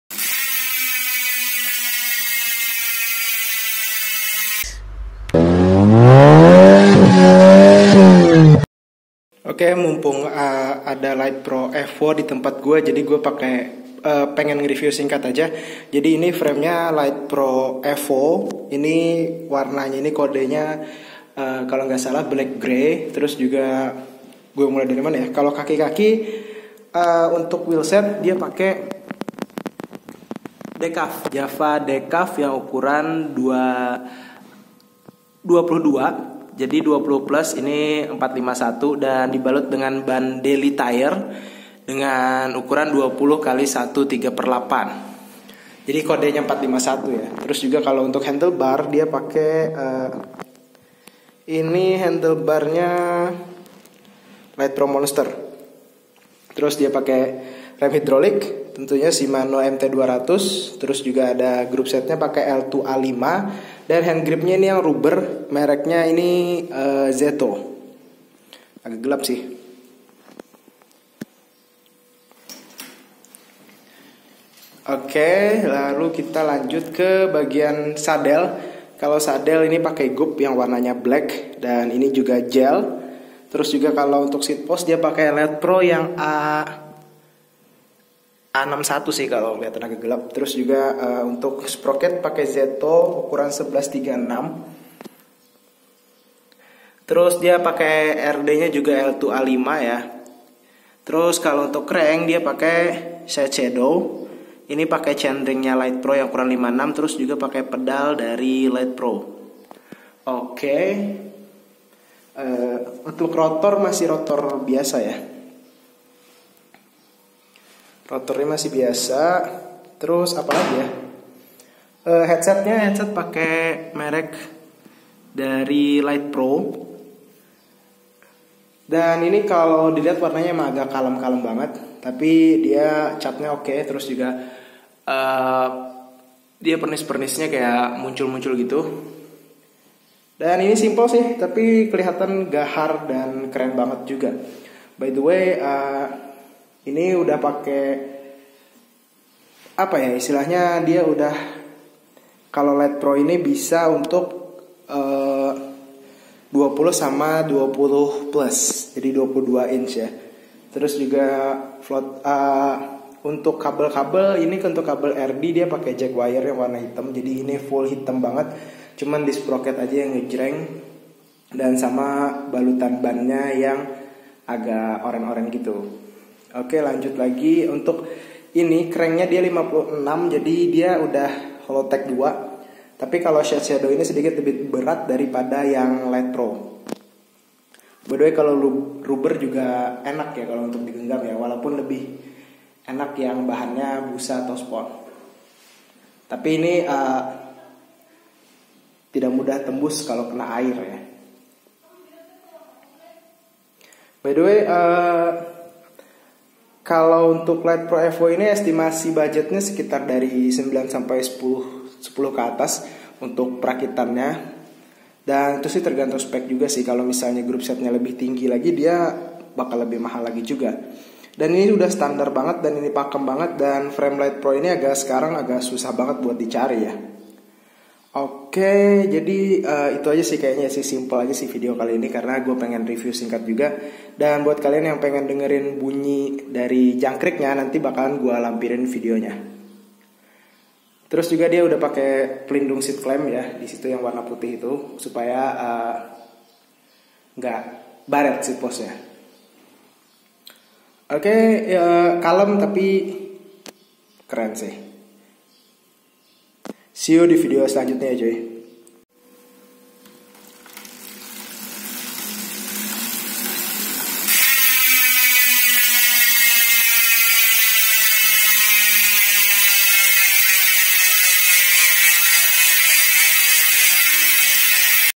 LITEPRO EVO SIMPLE KALEM TAPI MENARIK UNTUK DI PANDANG
Litepro evo review singkat

Frame Litepro Evo Chromolly
Handlebar Litepro Monster
Handgrip Ztto rubber

Brake set Shimano MT 200 Hidrolik
Headset litepro
Wheelset Decaf 451 Jangkrik

Tire Deli tire 451
Crank Syte Shadow Hollowtech 2

Chainring Litepro 56T
Sproket Ztto Gold 9 Speed 11-36T
Grup set Ltwoo a5 9 speed
Chain Vg Sport 10s
Pedal Litepro Bearing

Sadel Gub Gel

seatpost Litepro a61